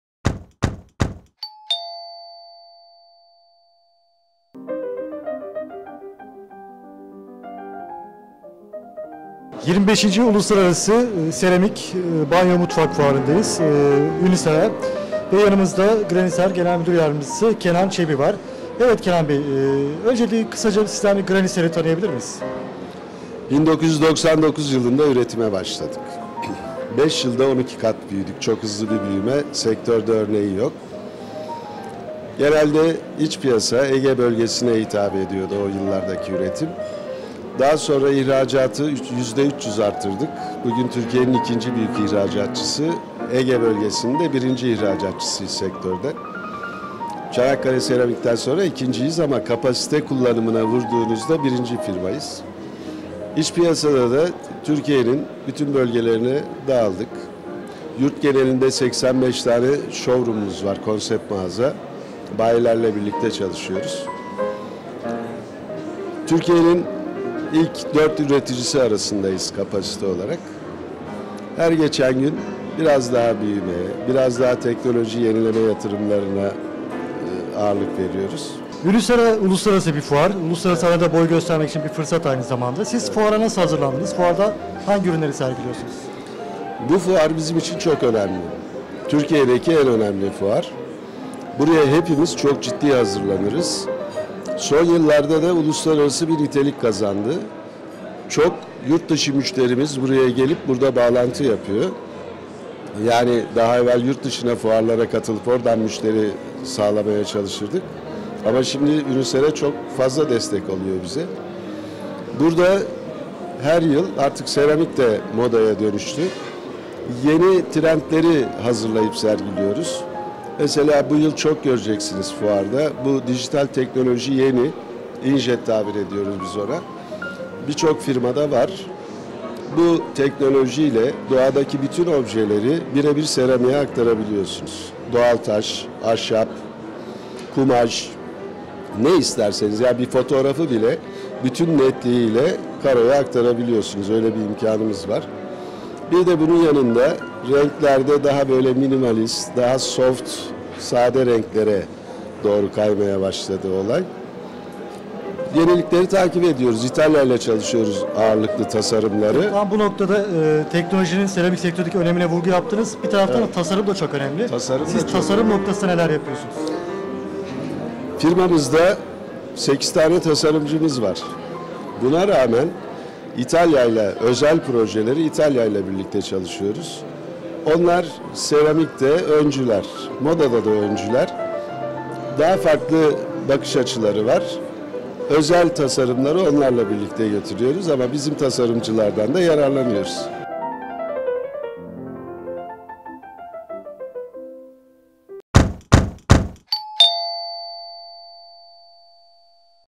Graniser'den yüzde 300'lük rekor büyüme
25. Uluslararası UNICERA Fuarın'da Graniser Genel Müdürü Yardımcısı Kenan Çebi emlakwebtv'ye konuştu. Çebi, seramikte teknoloji ve tasarımın ön plana çıktığını söyledi

6 Mart 2013 / emlakwebtv / Bülent Yoldaş
25. Uluslararası UNICERA Fuarın'da Graniser Genel Müdürü Yardımcısı Kenan Çebi emlakwebtv'ye konuştu. Çebi, seramikte teknoloji ve tasarımın ön plana çıktığını söyledi. Garaniser'in 1999 yılında üretime başladığını ve sektörde 5 yılda 12 kat büyüme kaydettiklerini belirterek, "Graniser'in büyümesinin sektörde örneği yok. Genelde iç piyasa, Ege bölgesine hitap ediyorduk. Daha sonra ihracatı yüzde 300 artırdık, bugün Türkiye'nin 2. Büyük ihracatçısı, Ege Bölgesinin de 1.Büyük ihracatçısıyız sektörde. Çanakkale Seramik'ten sonra ikinciyiz ama kapasite kullanımına vurduğunuz da birinci firmayız. İç piyasada da Türkiye'nin bütün bölgelerine dağıldık. Yurt genelinde 85 tane Şhowroom'umuz var konsept mağaza, bayilerle birlikte çalışıyoruz. Türkiye'nin ilk 4 üreticisi arasındayız kapasite olarak, her geçen gün biraz daha büyümeye biraz daha teknoloji yenileme yatırımlarına ağırlık veriyoruz" diye konuştu.

UNICERA'da teknoloji ön plana çıktı
UNICERA Fuarı'nı da değerlendiren Kenan Çebi, "UNICERA, uluslararası bir fuar. uluslararası arenada boy göstermek için iyi bir fırsat aynı zamanda.Bu fuar bizim için çok önemli, Türkiye'deki en önemli fuar. Buraya hepimiz çok ciddi hazırlanırız, son yıllarda da uluslar arası bir nitelik kazandı. Çok yurt dışı müşterimiz buraya gelip burada bağlantı yapıyor. Yani daha evvel yurt dışı fuarlara katılıp, orada müşteri sağlamaya çalışırdık ama şimdi UNICERA çok fazla destek oluyor bize. Artık seramik modaya dönüştü, yeni trendleri hazırlayıp sergiliyoruz. Bu yıl fuarda dijital teknoloji öne çıkyı. Bu teknolojiyle doğadaki objeleri birebir seramiğe aktarabiliyorsunuz: doğal taş, ahşap, kumaş, ne isterseniz... Bir fotoğrafı bile bütün netliğiyle karoya aktarabiliyorsunuz. Bir de bunun yanında renklerde daha minimalist daha soft sade renklere doğru kaymaya başladı olay" dedi.

Seramiğin olmazsa olmazı tasarım
Kenan Çebi Teknolojinin yanı sıra seramikte tasarımında öne çıktığına dikkat çekerek, "Firmamızda 8 tane tasarımcımız var buna rağmen özel projeleri İtalya ile çalışıyoruz. Onlar seramikte öncüler modada da öncüler. Daha farklı bakış açıları var. Özel tasarımları onlarla birlikte getiriyoruz ama bizim tasarımcılardan da yararlanıyoruz" diye konuştu.